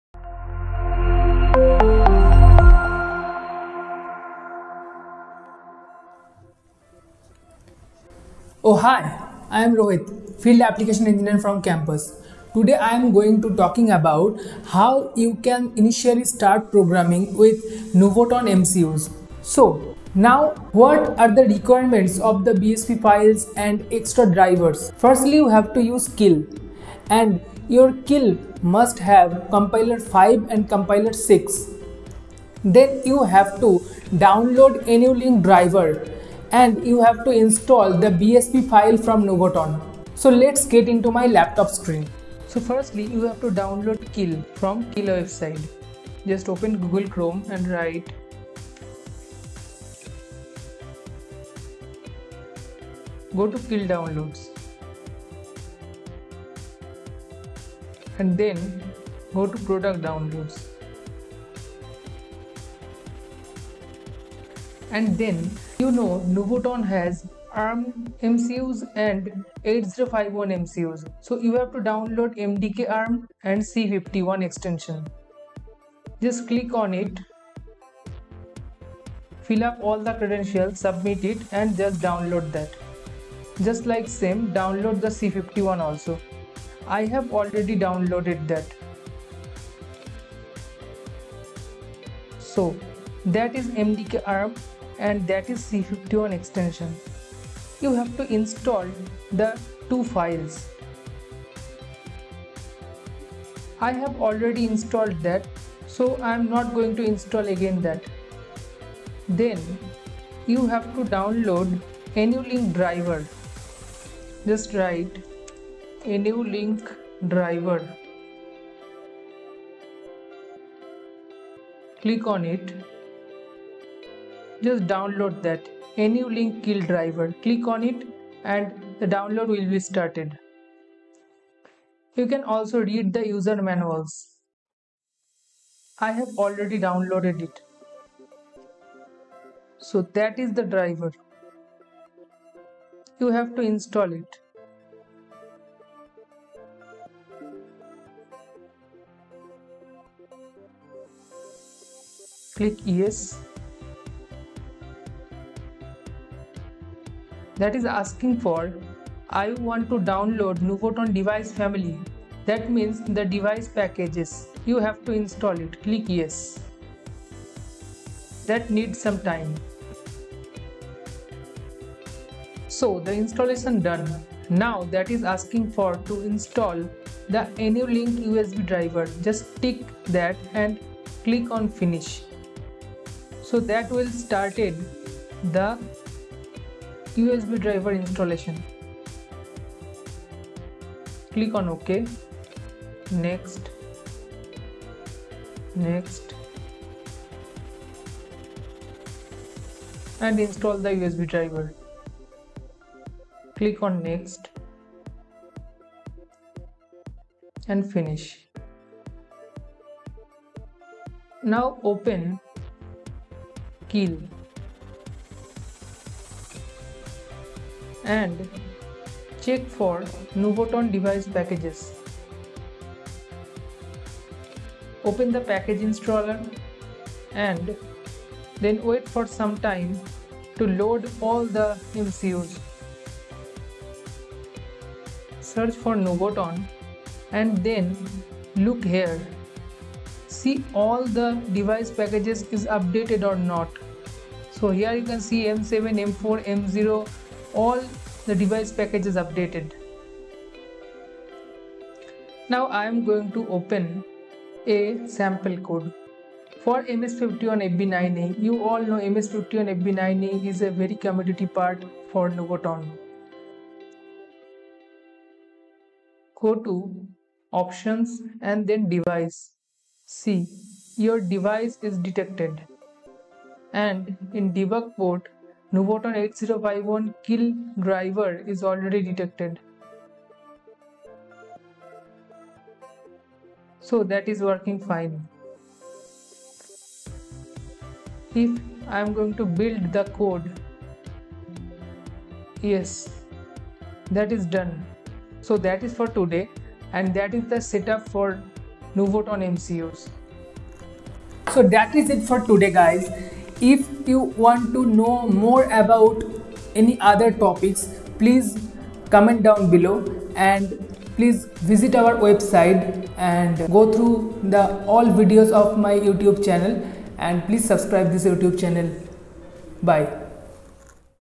Let's start with Keil MDK | NUVOTON | Arm Cortex | 8051 | MCU | Keil Uvison 5 | Campus Component
The Keil MDK is a complete software development environment for Cortex-M, Cortex-M0 processor-based devices. Keil-MDK specifically designed for microcontroller applications, it is easy to learn and use, yet powerful enough for the most demanding embedded applications. Let's start with keil-MDK.

Campus Component